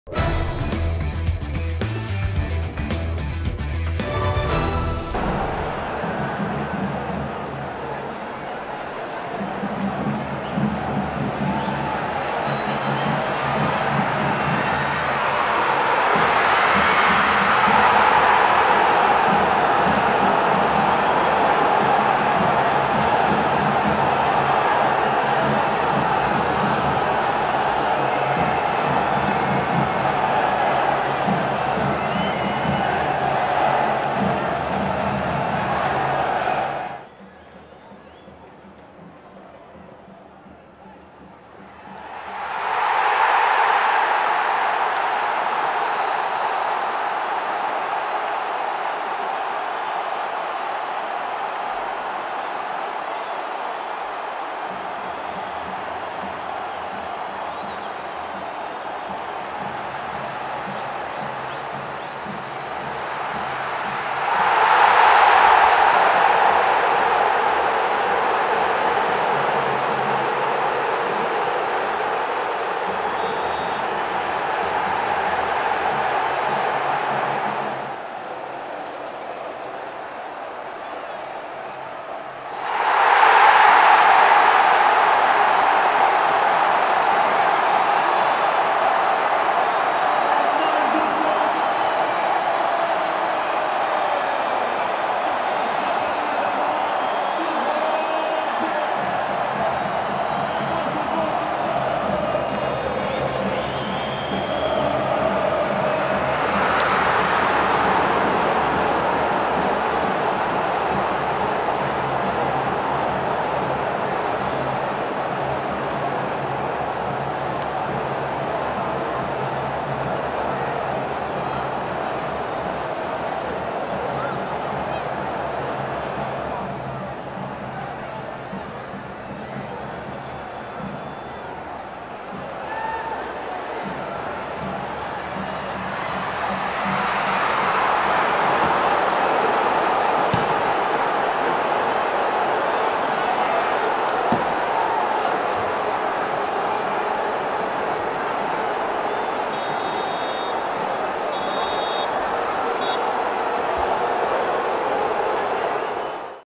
Vitória vence o Botafogo por 3 x 2 pelo Campeonato Brasileiro
Cliente: UOL
Serviço: Captação de imagens e edição
Produção: Léo Nunes
Realização: NNS FILMES ®